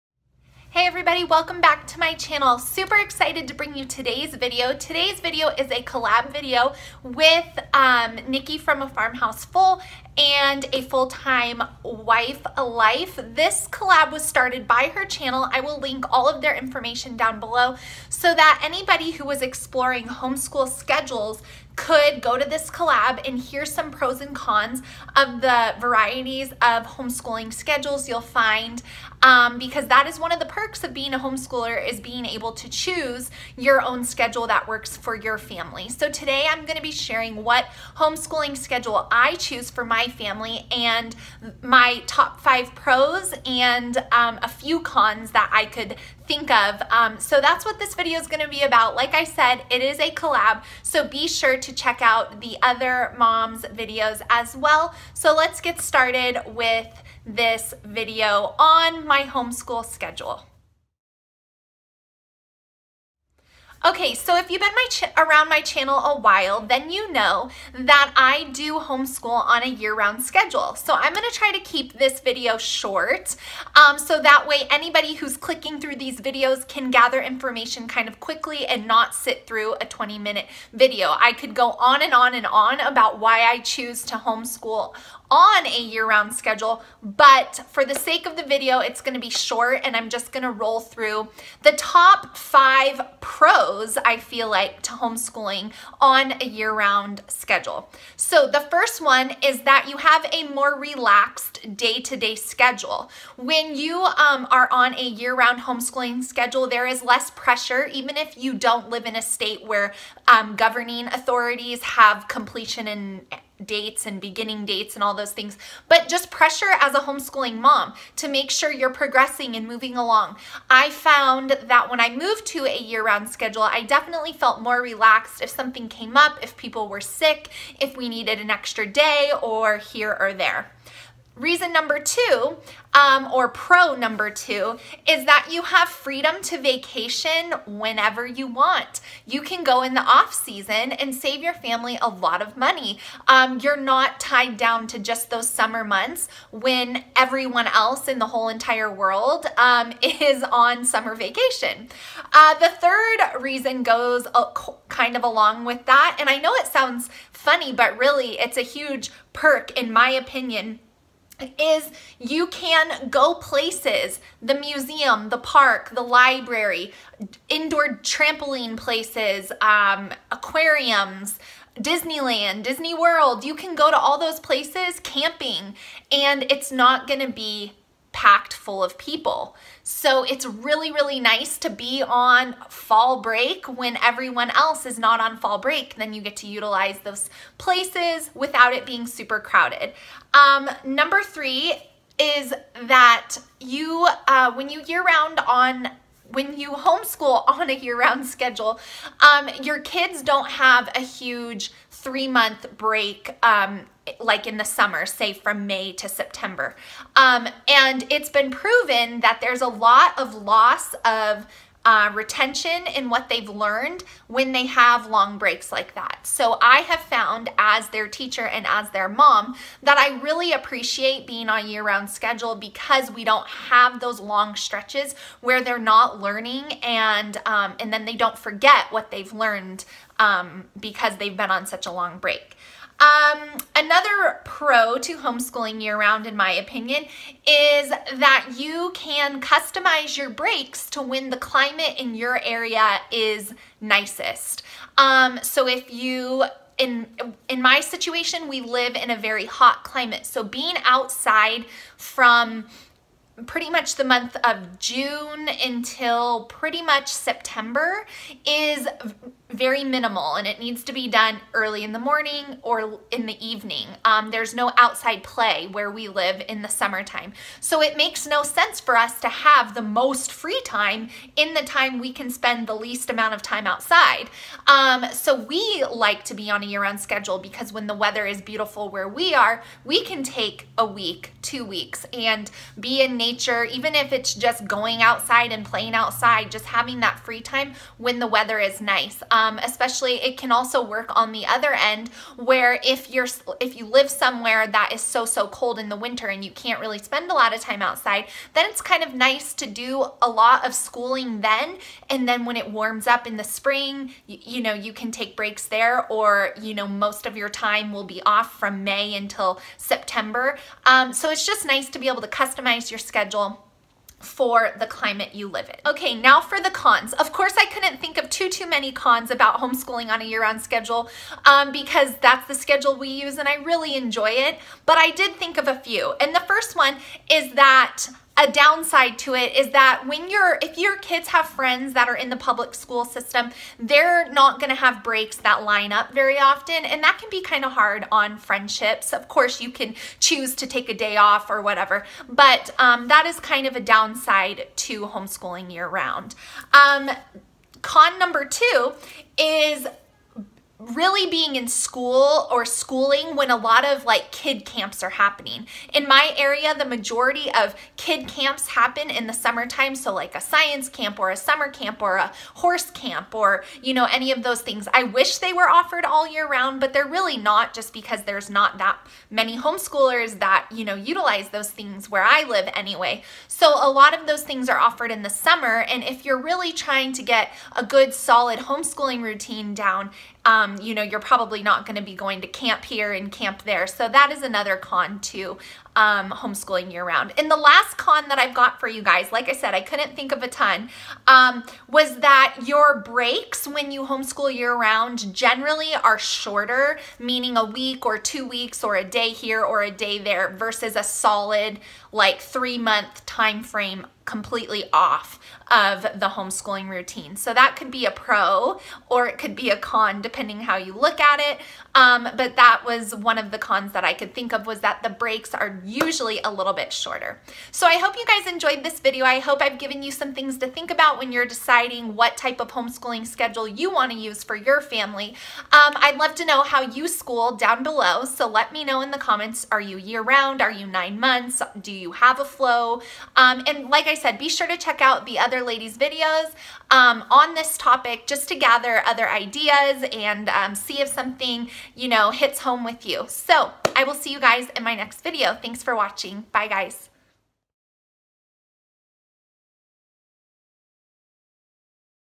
HOMESCHOOLING SCHEDULE OPTIONS||YEAR ROUND SCHOOLING
SO Excited to be bringing you this video today! This is a collab video with two other amazing home educating mamas:

We are sharing our schedule choices for our homeschools' and a quick overview of some awesome things and maybe a few not so awesome things about doing school this way! I hope this video helps anyone out there considering what schedule system they hope to follow!

My favorite things

~Ashlee